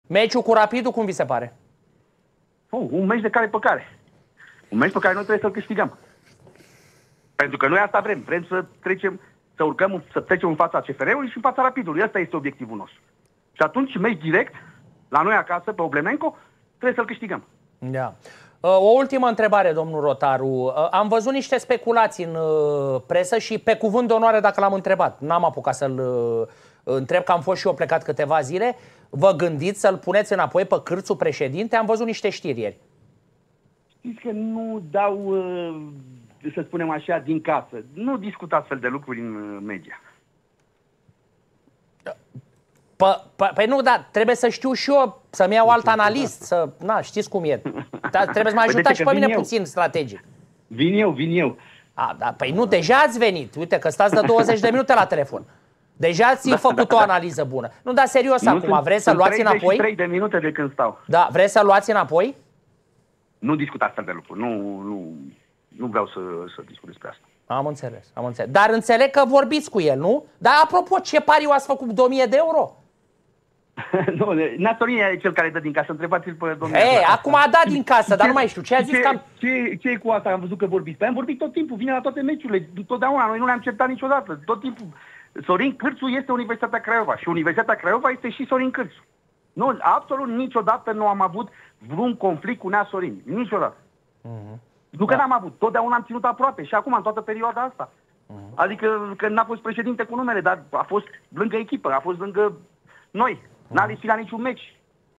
Mihai Rotaru, OBIECTIV CLAR in DERBY-UL U Craiova - Rapid: “E CARE PE CARE”
In direct la FANATIK SuperLiga cu Horia Ivanovici, patronul Mihai Rotaru a spus care este obiectivul Universitatii Craiova la derby-ul cu Rapid. Ce asteptari are Mihai Rotaru de la meciul U Craiova - Rapid